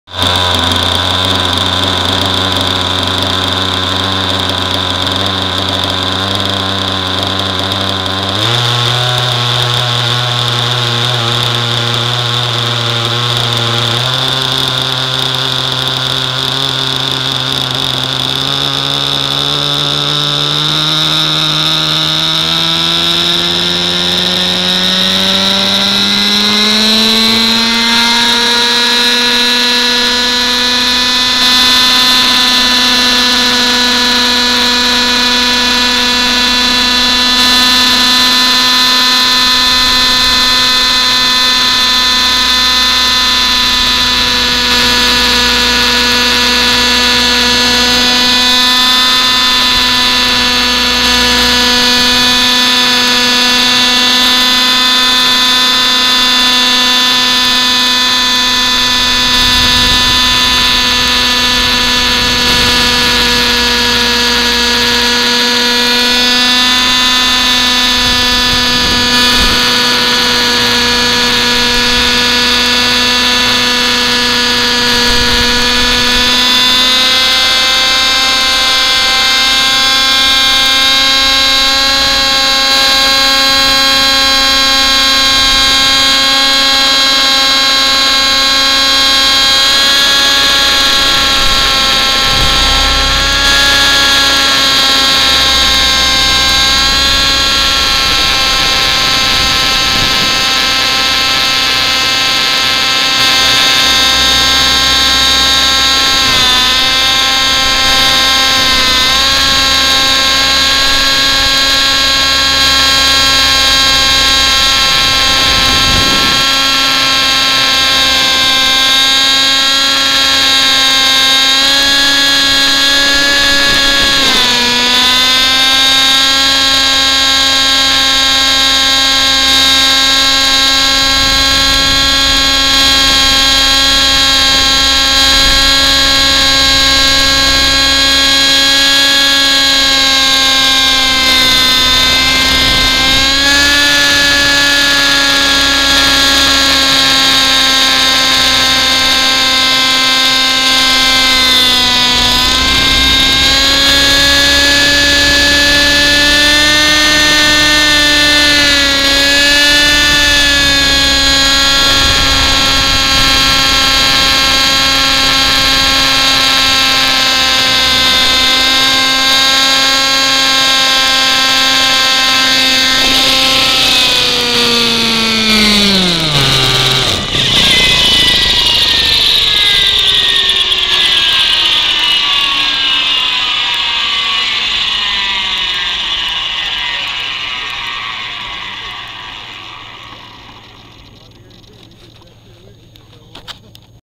Raptor 50 Titan Onboard Video Facing Rear
This time I mounted my camera mount backwards so the camera is behind the skids and you can see the tail and tail rotor throughout the flight.  This time some inverted flying.